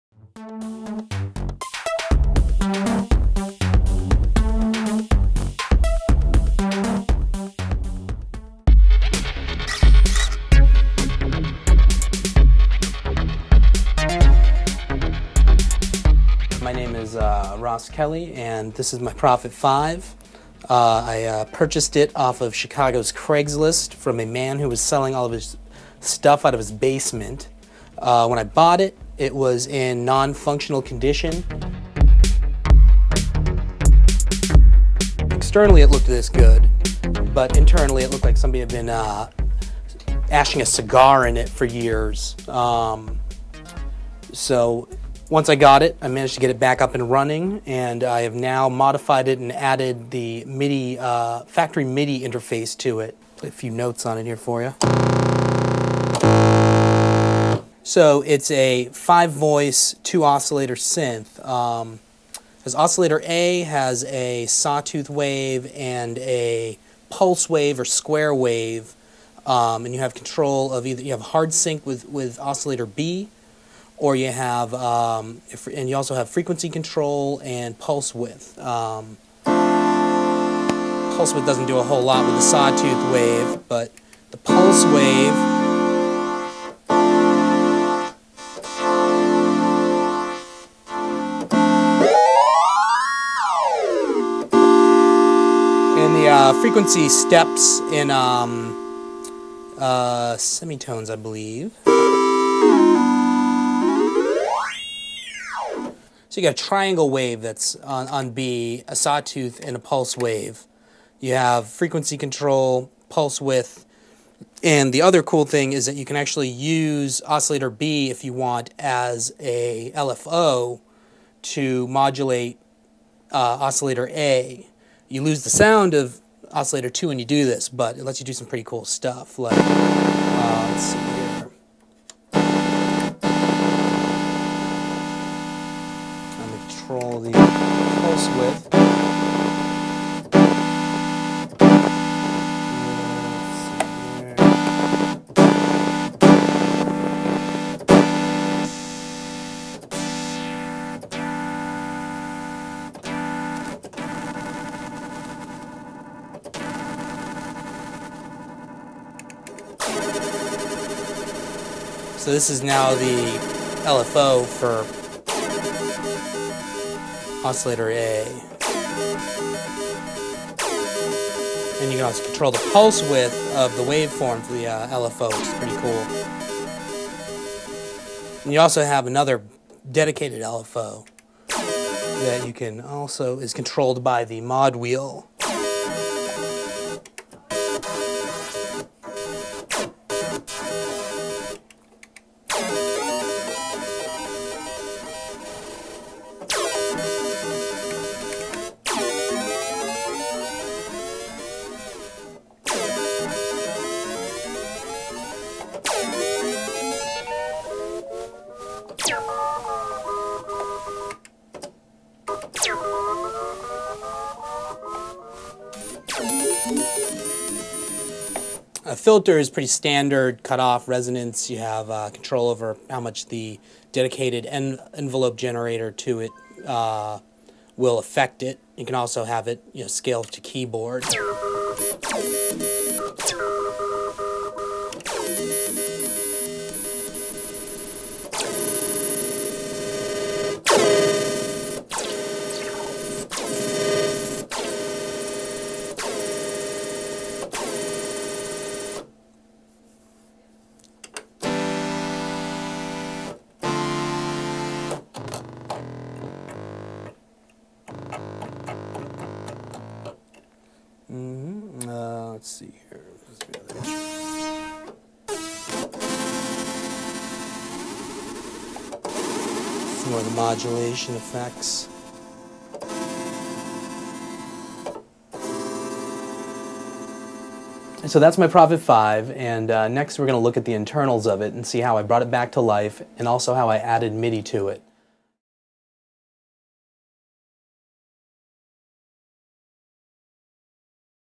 Sequential Circuits Prophet 5: Adding MIDI Compatibility
Ever been searching for gear on eBay or craigslist for, let's say, a Prophet 5, and you come across a price that's just to good to be true, and then upon further inspection you realize that some dude is selling a non-functioning unit, which is why you can actually afford it, so you throw your hands up and curse the heavens for knowing nothing about soldering or even electrical engineering?

Chicago DJ / Producer Ross Kelly had this exact experience, except he actually knew how to make his basement-find fully-functional once more. Not only that, be he added MIDI compatibility in the process. Show-off. In this video he runs through the multitudinous and now-working sounds of his Prophet 5. See more on Gearwire.com.